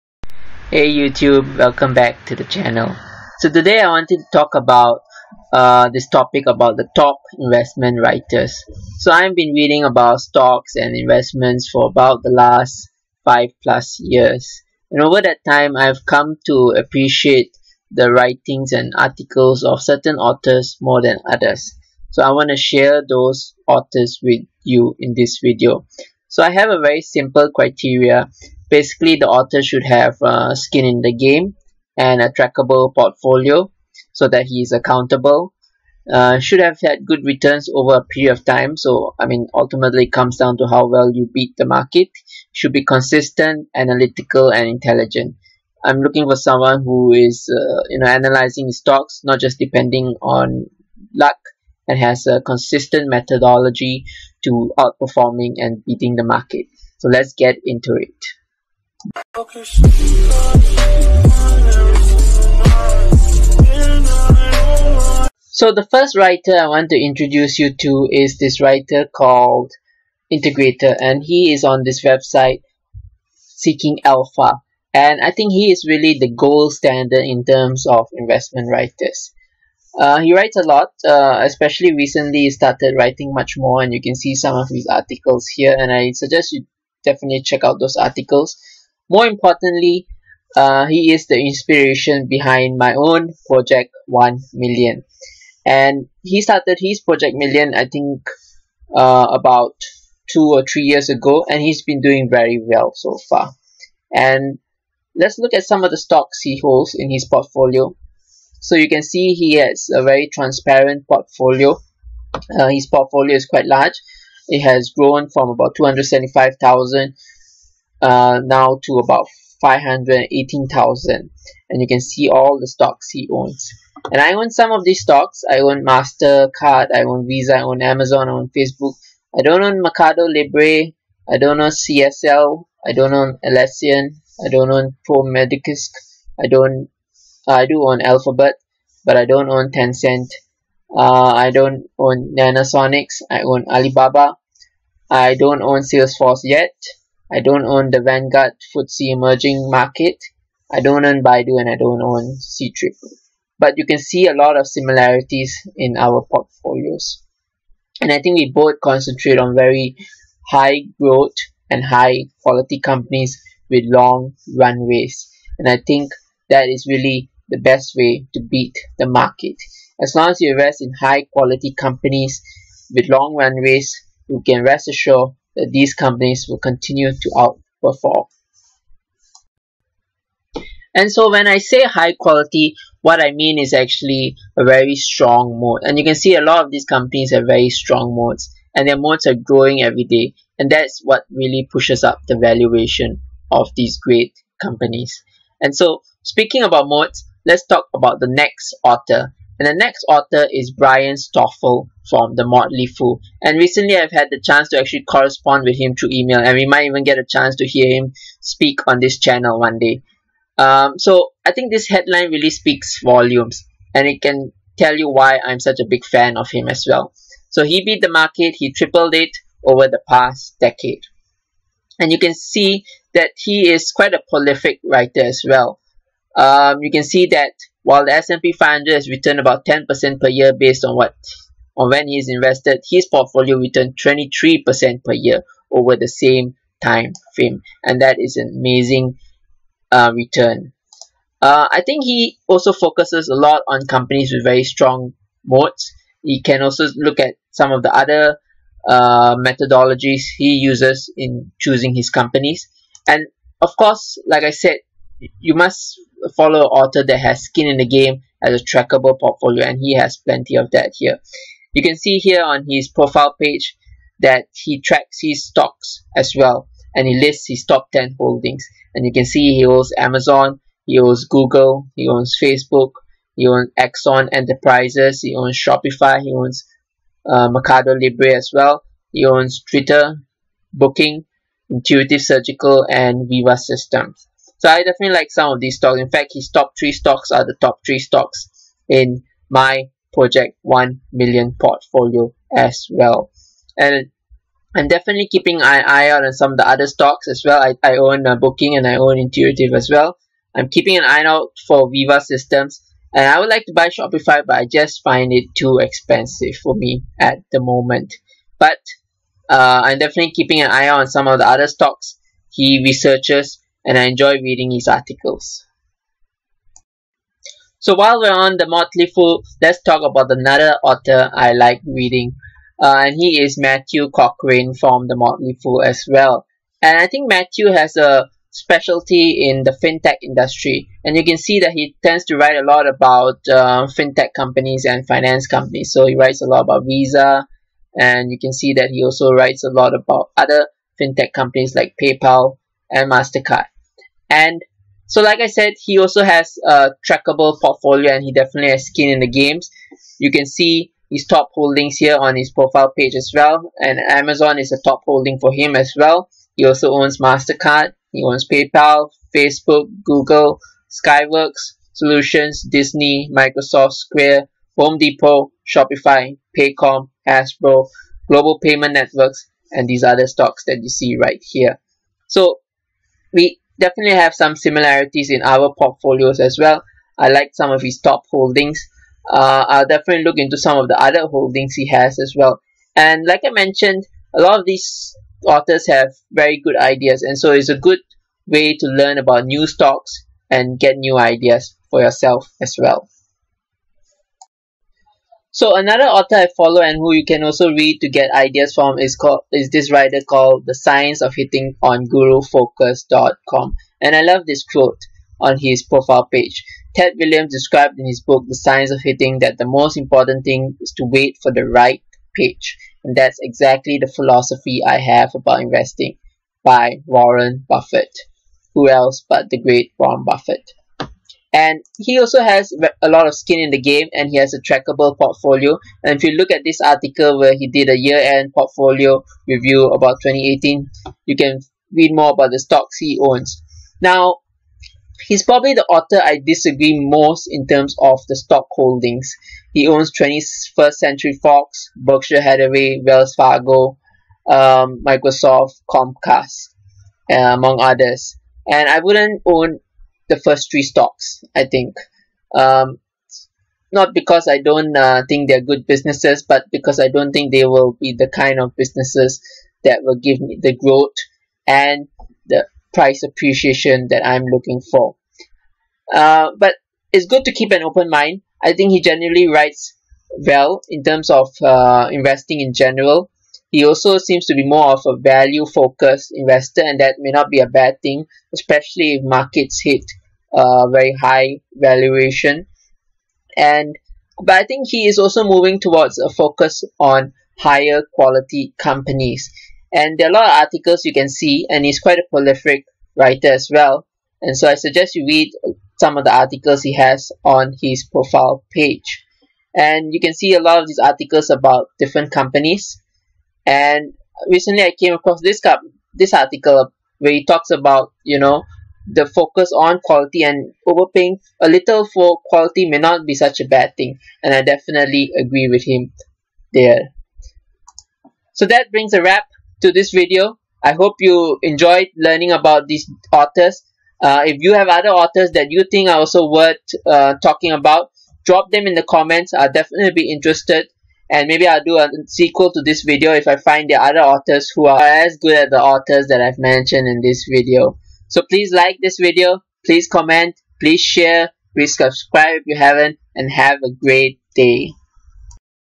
Best Investment writers to learn from to beat the market
In this video I discuss some of the best investment writers I have found over the years and I share them with you.

This is the link to the article by Brian Stoffel in the video

If you would like to interact or follow articles I read, you can do so here :